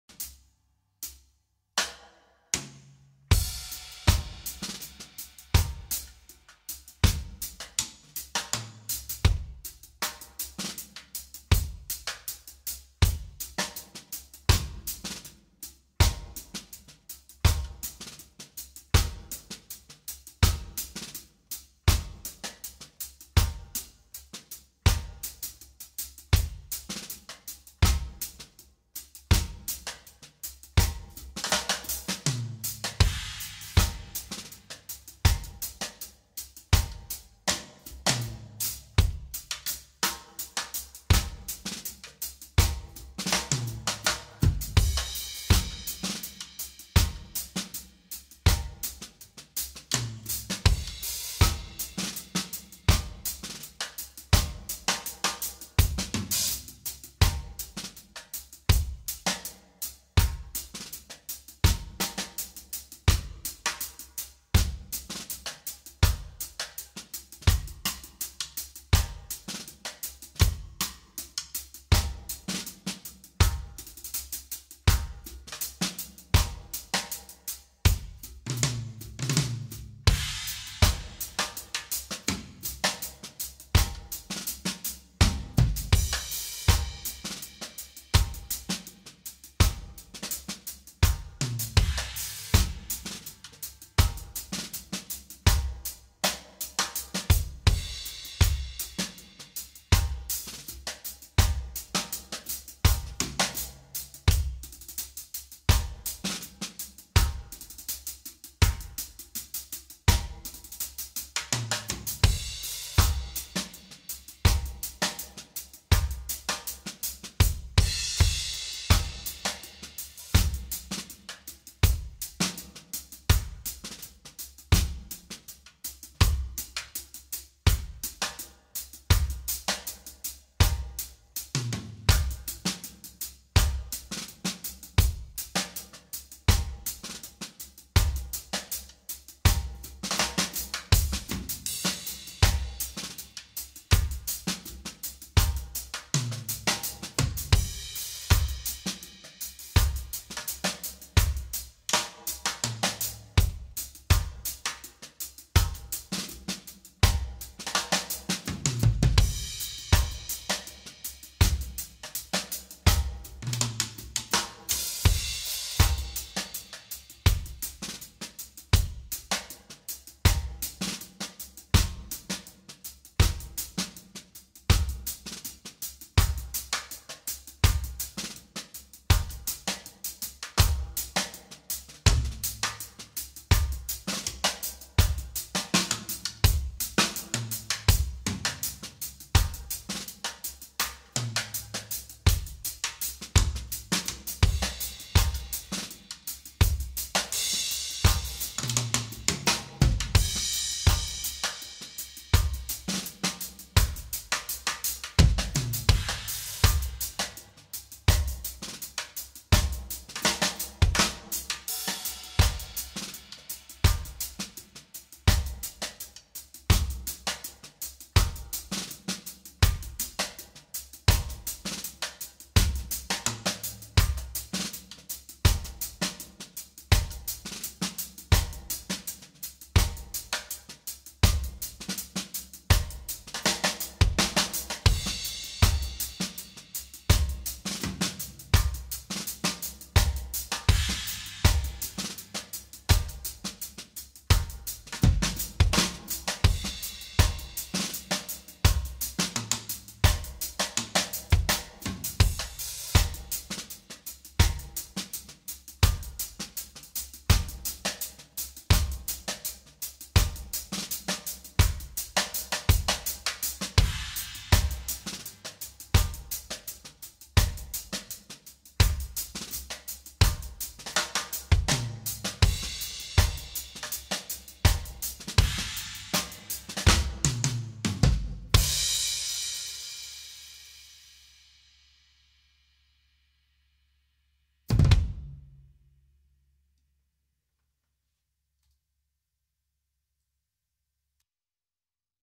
Drum Beat Reggae Medium Tempo backing track
To share your music, record your original song over this beat and add a link to your finished work in the comments. Your energy will elevate. This is a medium tempo Reggae track that goes for about 5 minutes. Why haven't I done this before? Who knows? I love playing Reggae. Ok, I'm not the best in the world or anything, but hey, fun sounds like fun.
Snare drum used in this recording is "Pearline", my 1963 14" x 5" White Marine Pearl maple shell Ludwig Classic Snare
If you would like a custom recording for an original project, get in touch via the comments Stay cool, Peace, out!